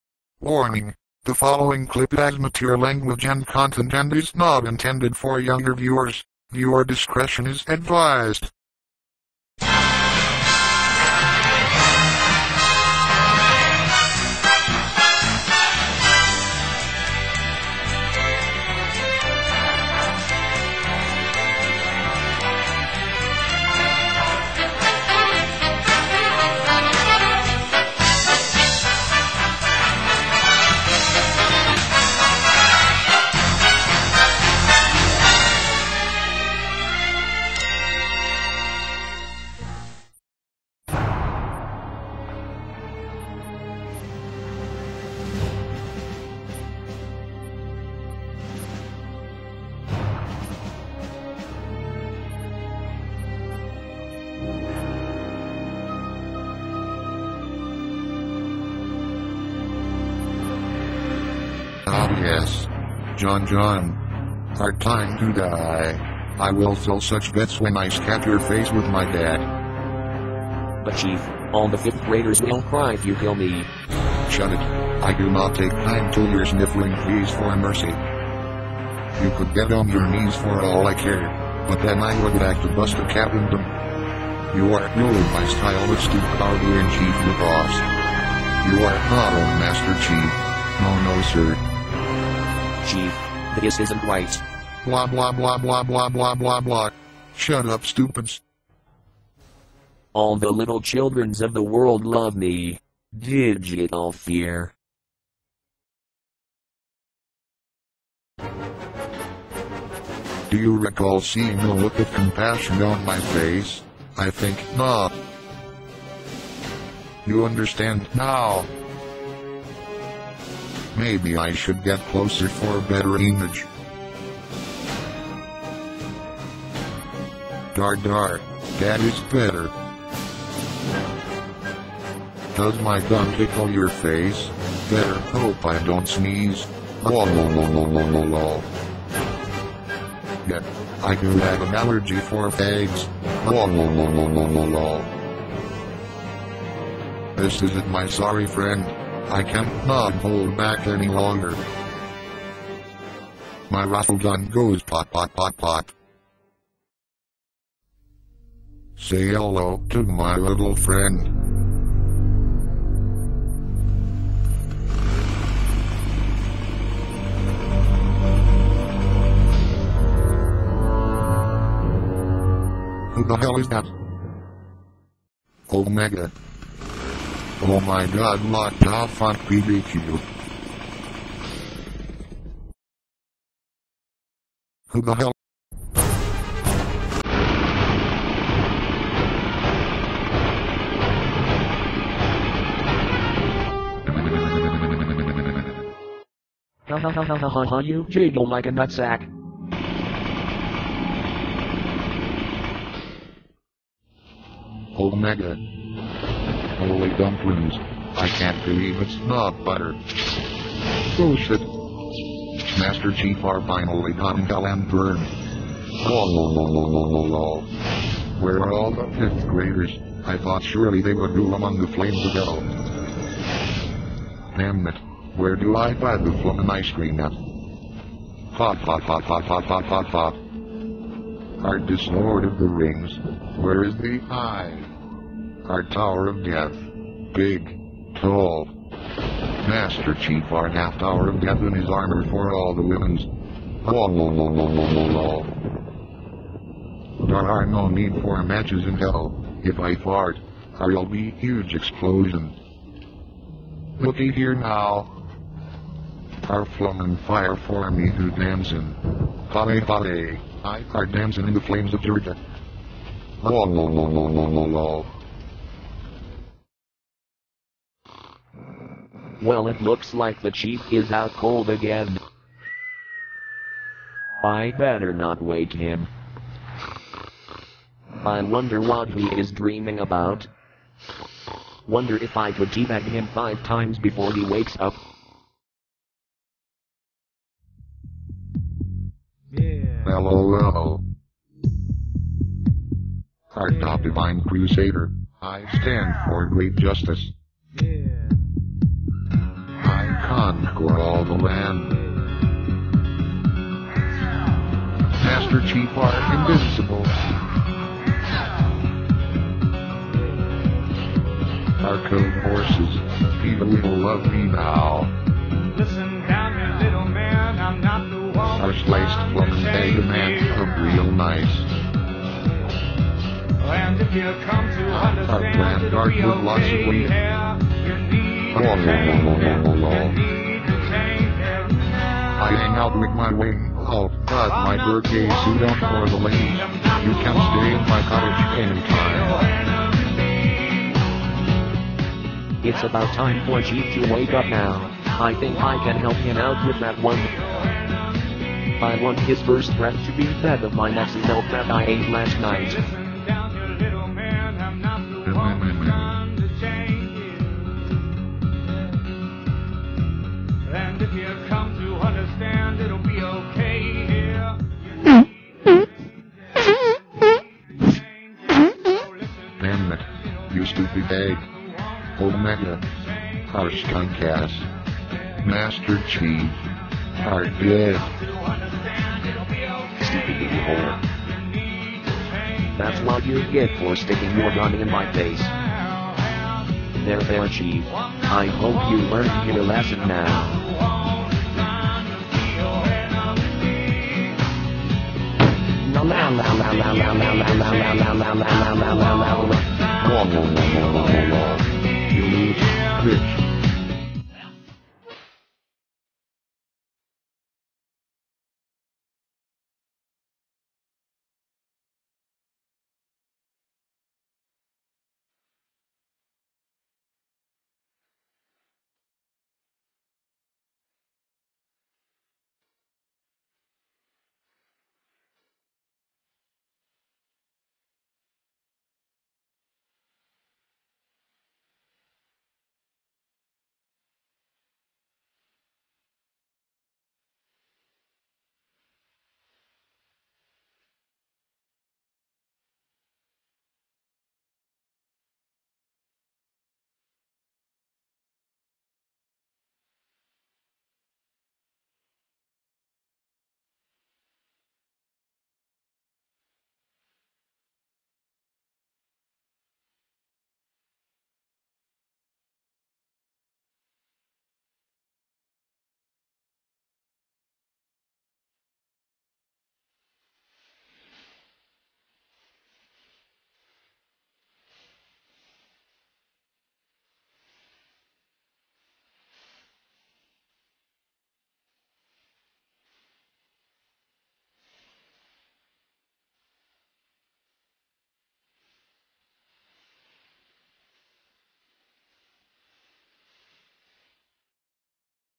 Master Chief Is Invincible: Episode 4 - "Flamin Fire"
MCII: Episode 4 - "Flamin Fire"

Master Chief finally has Jon Jon at gunpoint and is about to pull the trigger. Will Jon Jon live? Marcus Fenix makes a guest appearance.